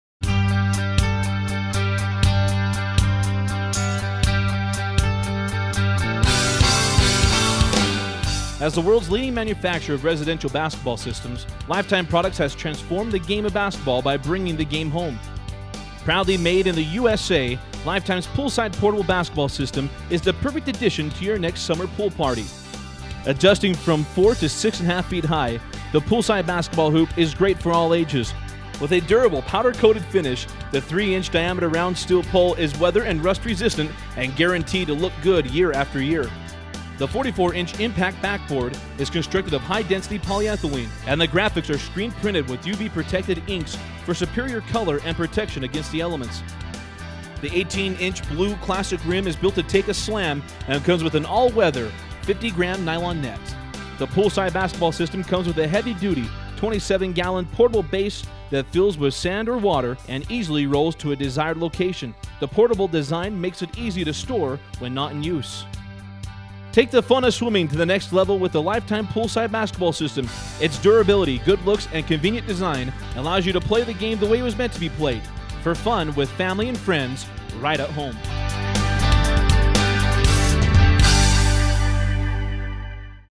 Lifetime Portable Swimming Pool Hoop 1301 - KitSuperStore.com
The poolside water basketball hoop is designed with a 44" Impact backboard of heavy-duty plastic and a telescoping backboard height adjustment feature. Combine swimming and basketball into one awesomely fun activity. The 44" backboard is constructed of weather-resistant polyethylene plastic and can be raised or lowered from 4 - 6.5 ft. The Lifetime Poolside Court Hoop has a 27 gal. portable base which is filled with sand or water for stability and rolled to any desired location around your pool. Shoot hoops in the pool while you swim!

Call our expert staff 7 days a week at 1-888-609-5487